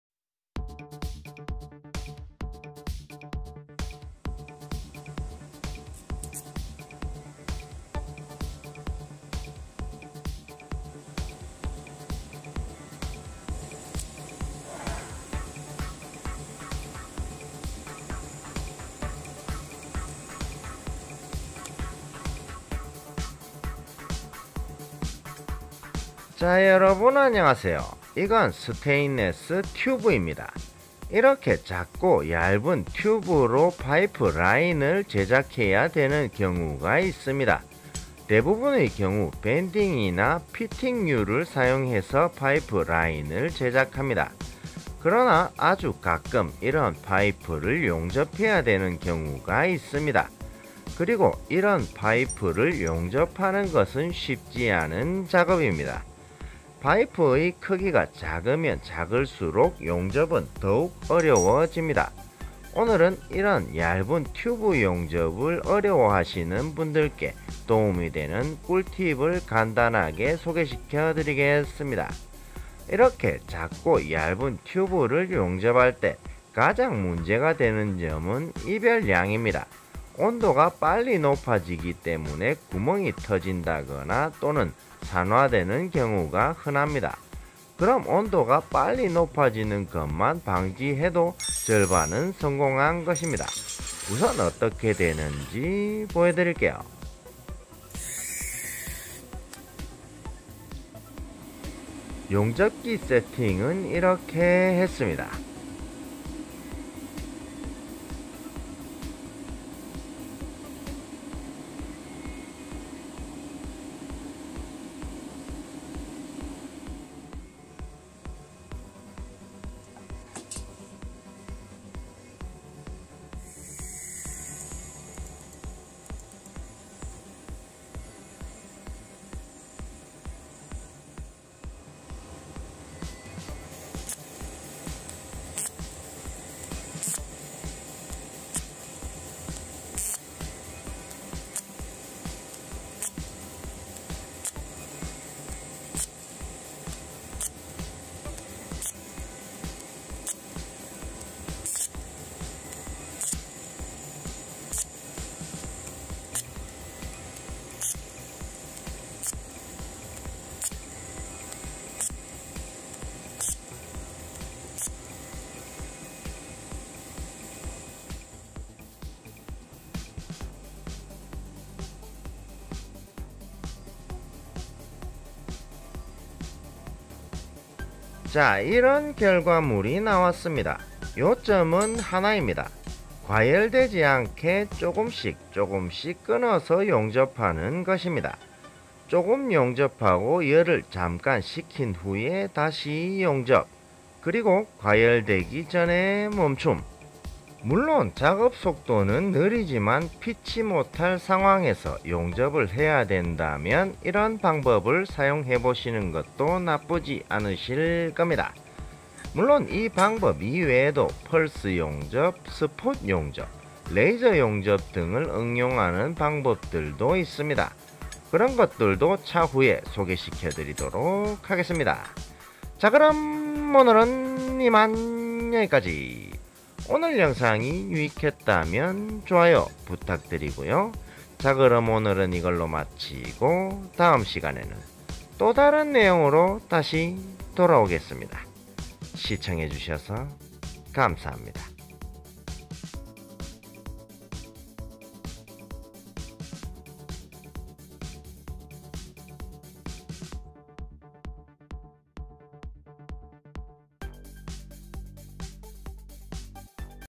Few know this way! Amazing TIG Welding Tricks That Only Professionals Use
For information on welding machines and peripherals, you can get more accurate answers by contacting the manufacturer.

Hello everyone.
When making pipelines with small diameter tubing, bending or fittings are used most of the time.
However, very occasionally it is necessary to weld small bore tubes.
Today, I will briefly introduce some tips for small-diameter tube welding.
In the future, we will also introduce the application of pulse welding, cold welding, and laser welding.